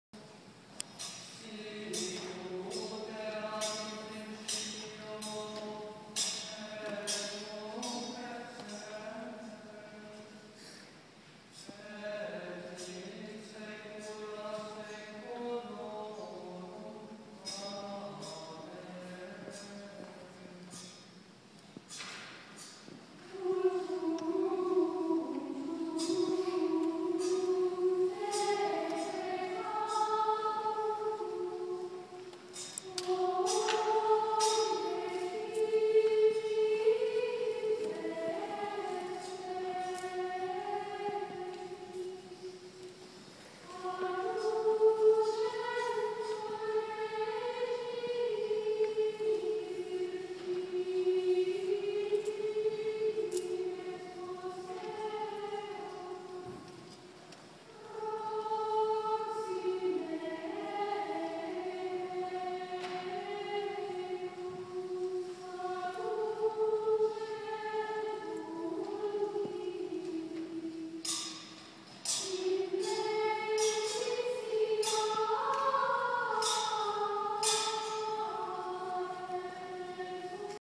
Pontifical High Mass, St. James Cathedral, Brooklyn, NY
Bishop Athanasius Schneider offers the Holy Mass according to the "extraordinary form" of the Latin Catholic rite in St. James Cathedral, Brooklyn, New York, USA, 5 January 2012.